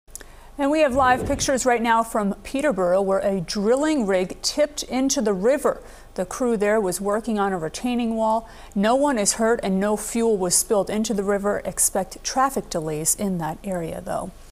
Traffic delays expected in Peterborough where drilling rig tipped into river
The crew there was working on the Route 202 retaining wall project at the time. No one was hurt.